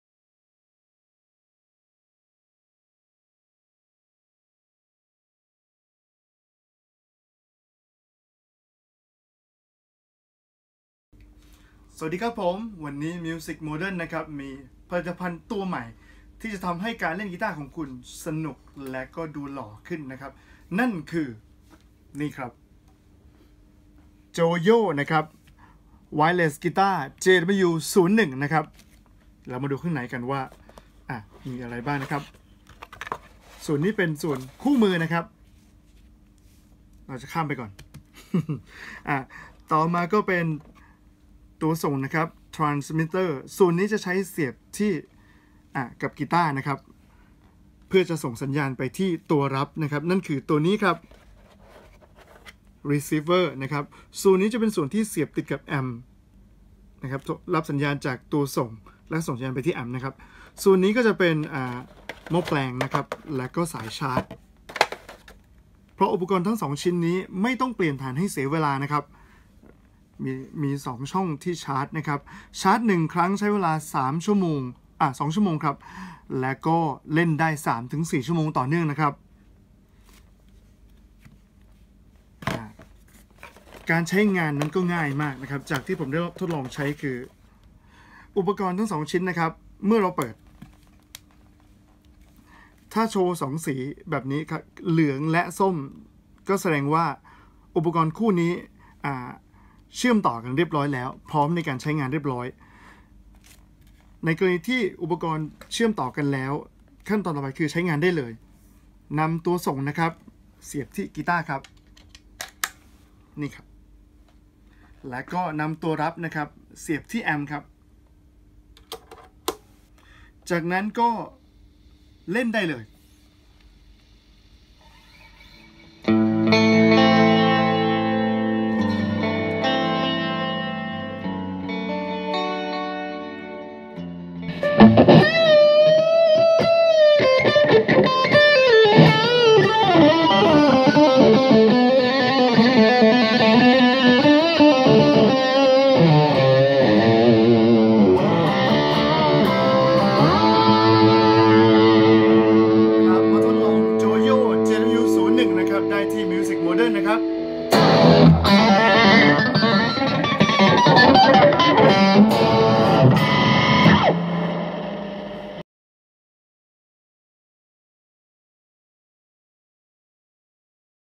ขาย ไมค์ลอย wireless guitar joyo jw- 01 ไวเลสกีต้าร์ คุณภาพดีราคาถูก โจโย ราคาถูก
musicmodern ขอแนะนำสินค้าใหม่ที่จะทำให้คุณใช้งานกีต้าร์โปร่ง หรือกีต้าร์ไฟฟ้า ได้สะดวกยิ่งขึ้น นั้นคือ  ไวเรส หรือ อุปกรณ์ไร้สาย สำหรับกีต้าร์ ซึ่งสามารถเล่นกีต้าร์ในระยะไกลๆ ได้ โดยไม่ต้องใช้สาย โดยคุณภาพเสียงยังคงเหมือนเดิม คุณสัมบัติที่สำคัญ คือ ใช้งานง่ายมาก กดเปิด สามารถใช้งานได้ทันที สนใจสินค้า สามารถติดต่อได้ที่  ร้าน musicmodern สีลม หรือ สอบถามข้อมูลเพิ่มเติมได้ที่ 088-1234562 คุณวิค Line ID : musicmodern